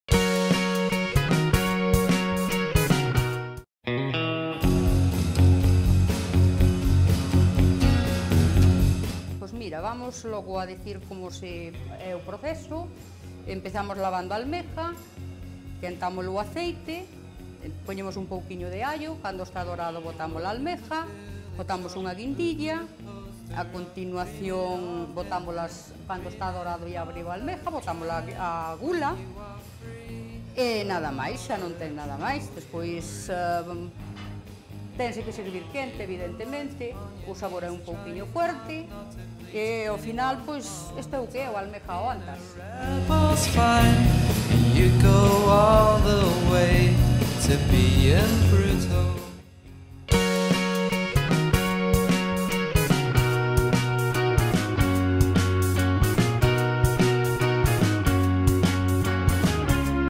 VII Concurso de Tapas de Lugo. Tapa do Antas de Ulla
Elaboración da tapa coa que compite o restaurante Antas de Ulla no VII Concurso de Tapas de Lugo.